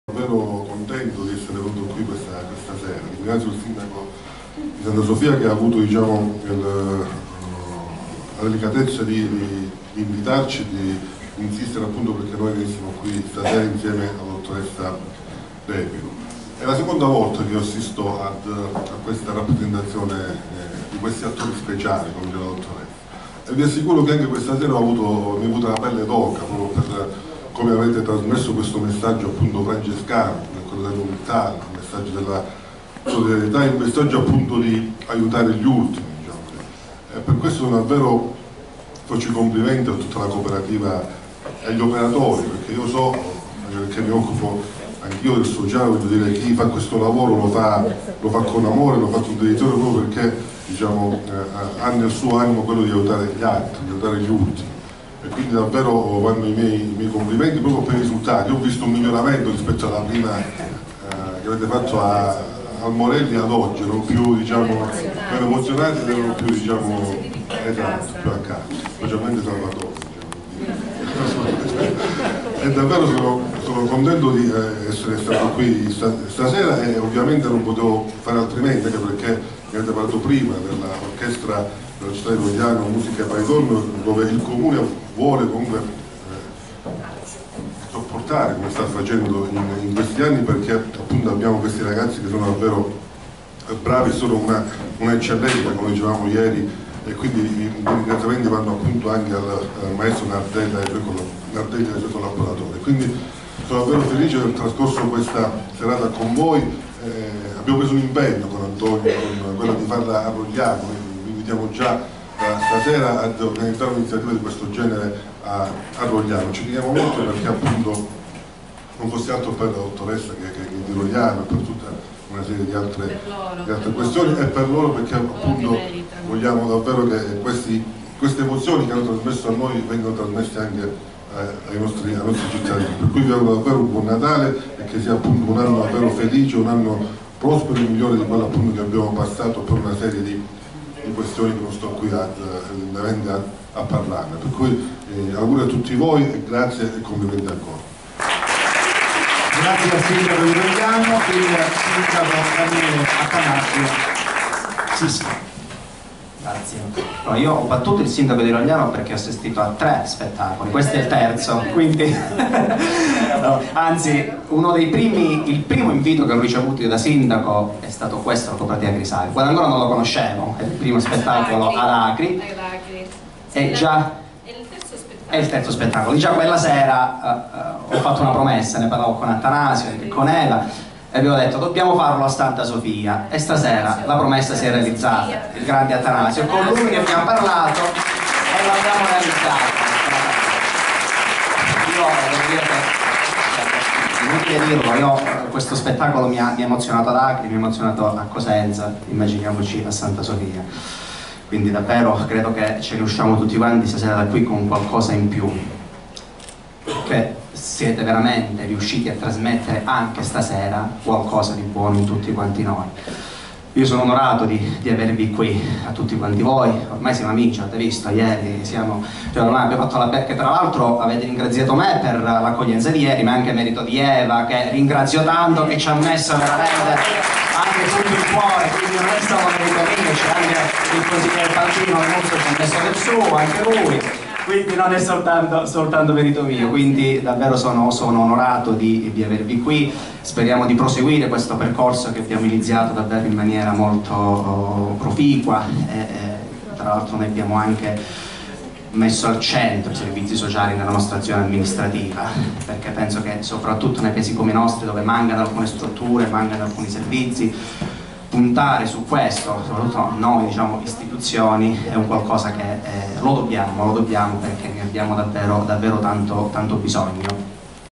I sindaci di Rogliano e Santa Sofia d'Epiro lodano spettacolo teatrale "Francesco il giullare di Dio
I sindaci di Rogliano Giovanni Altomare e Santa Sofia d'Epiro Daniele Sisca lodano spettacolo teatrale "Francesco il giullare di Dio.